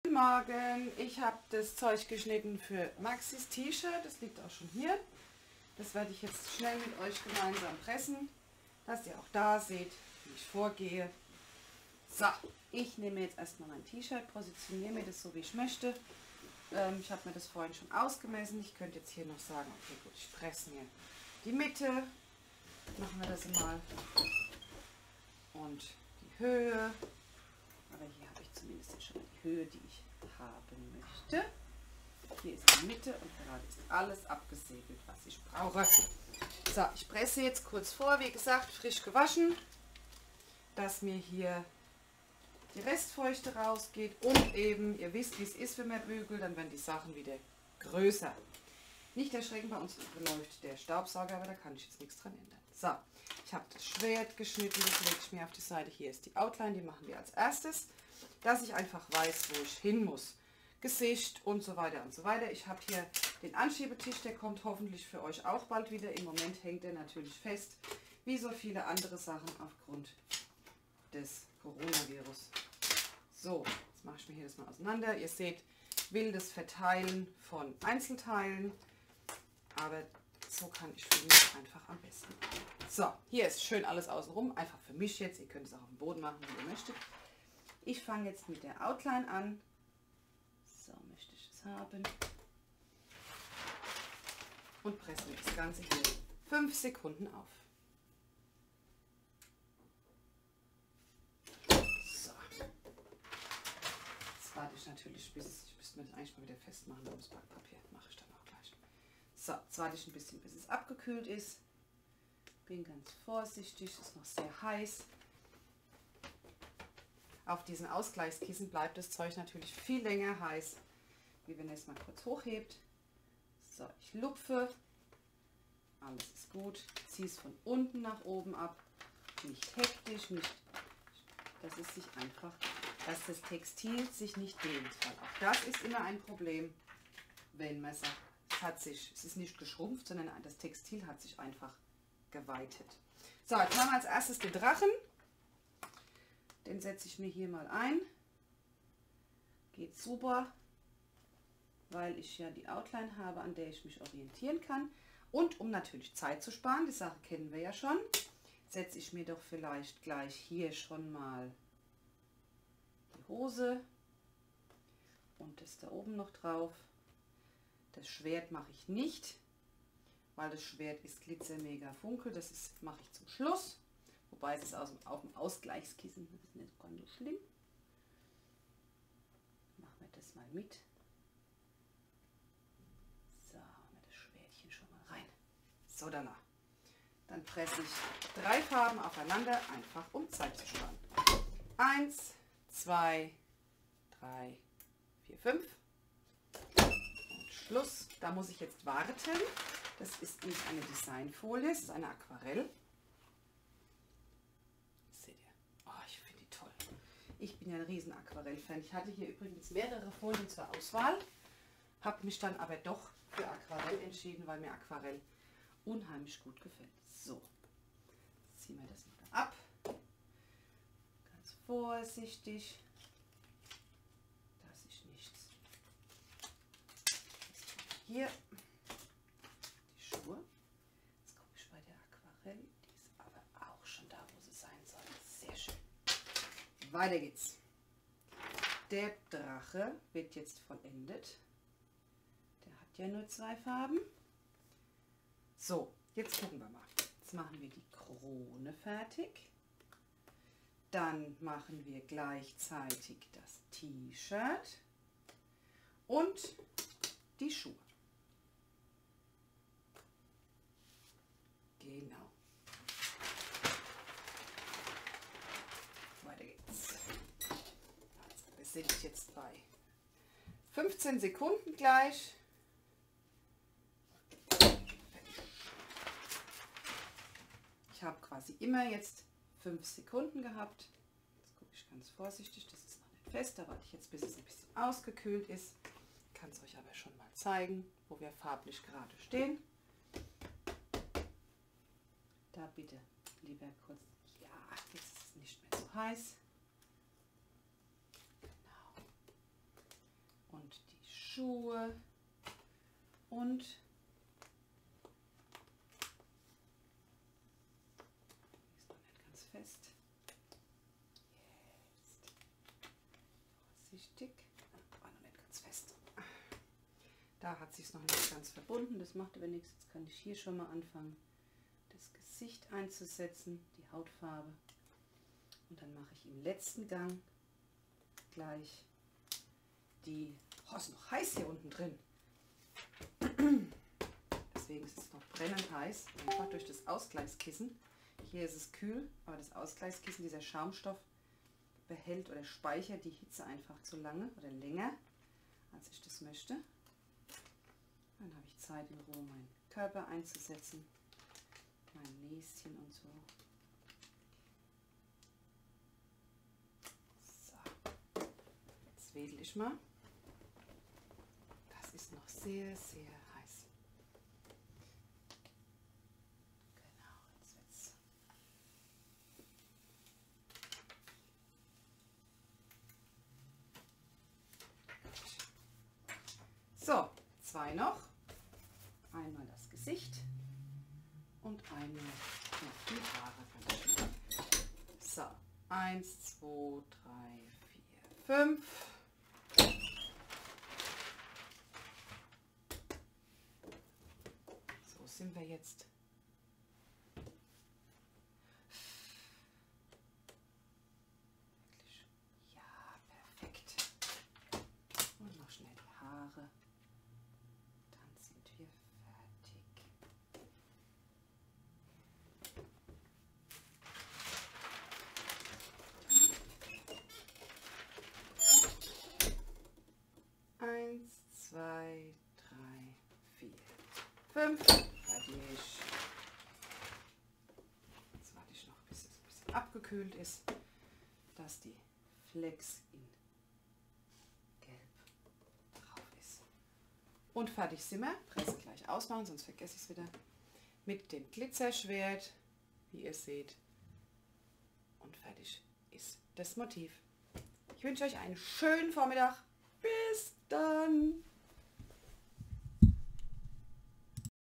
Maxis Shirt - Teil 2 der Sprechstunde vom 12.03.2020
Folien Flex S, Eigenproduktion Flex Aquarell und Glittermegafunkel
Datei von Emmapünktchen

Teil 1 der Sprechstunde folgt auch noch heute Vormittag.
LG Eva